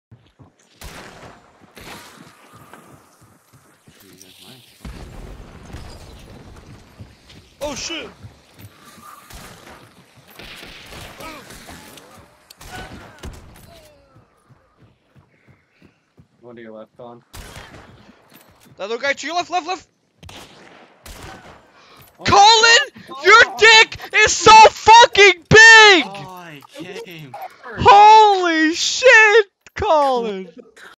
I Clutched Up For Once w/ @Sovuryn Being The Ultimate Hypeman
Thanks to daddy Ryan for recording because I don't do what I tell people to do...